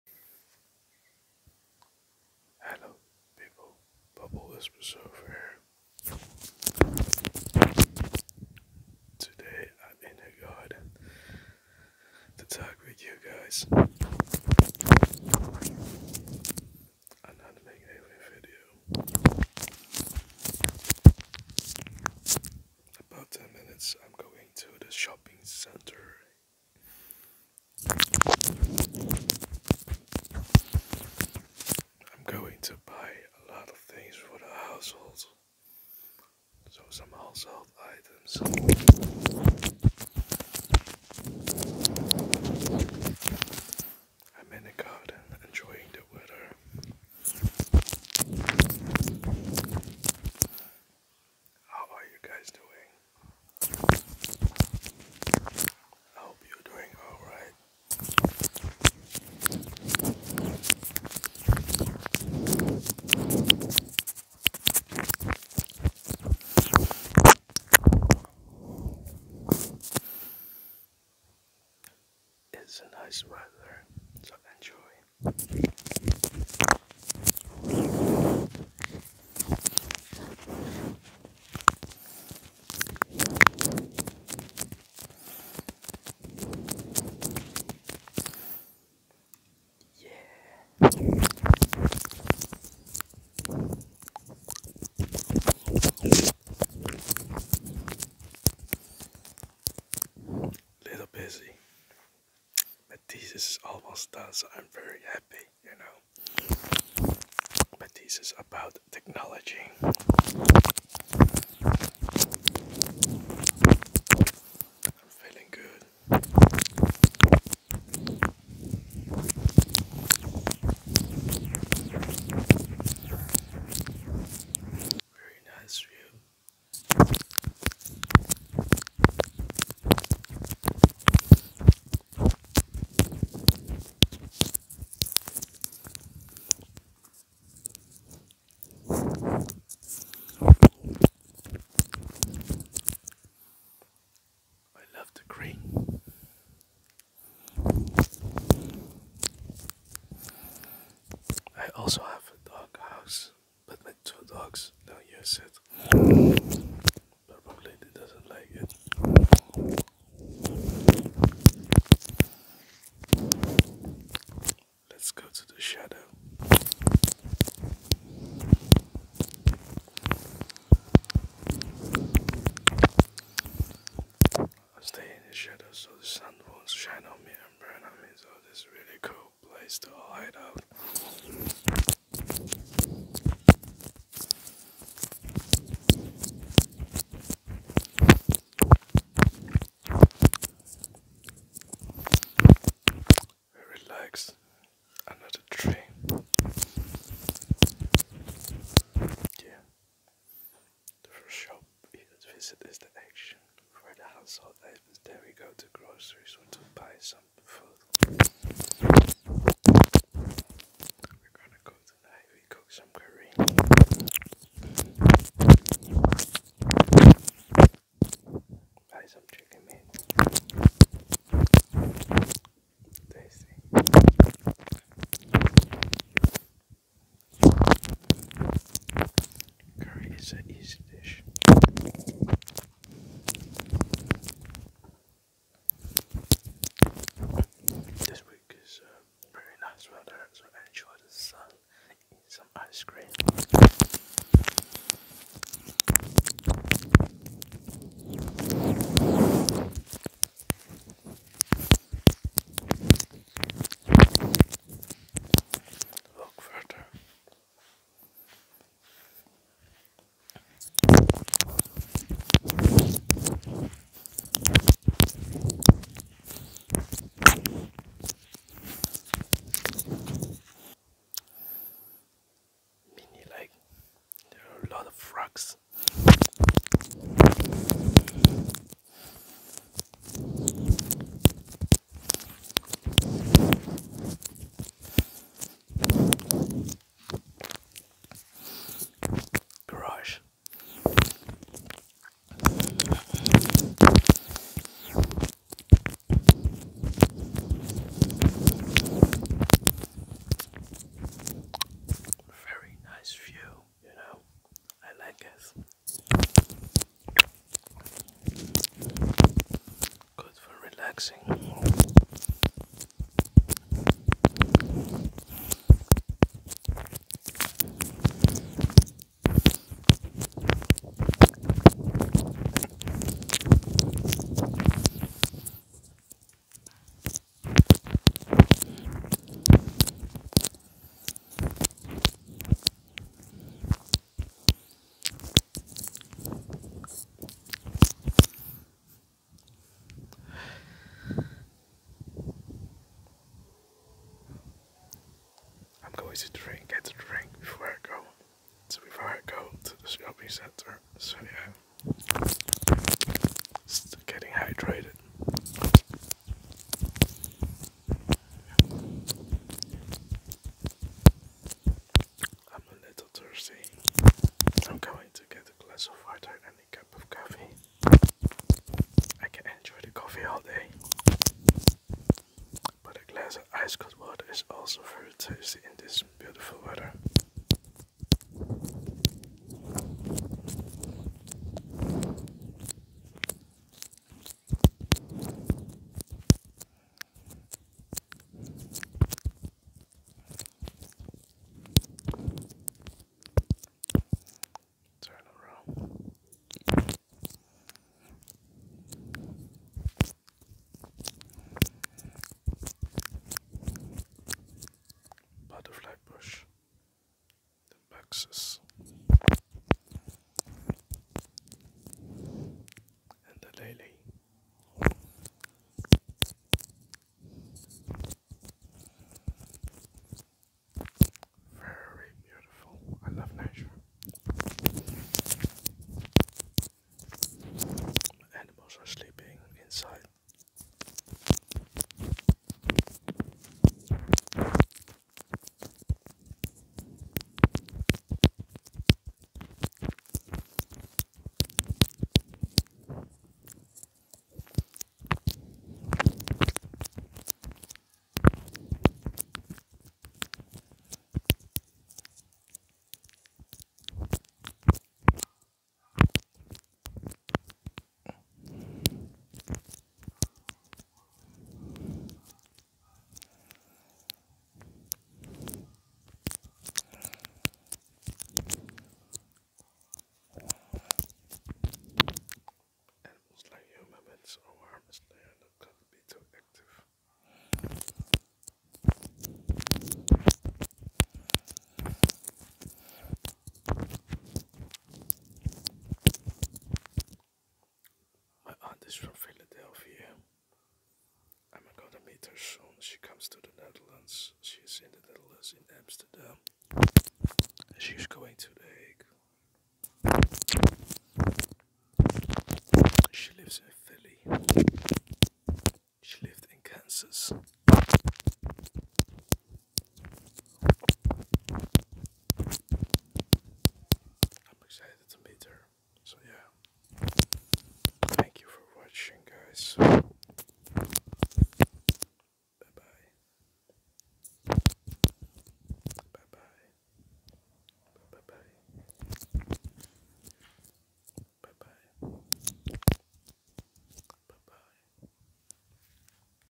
ASMR Mic Nibbling In The Garden ~ Intense Mouth Sounds
In this ASMR video I'm doing mic nibbling in the garen to produce intense mouth sounds for relaxiation.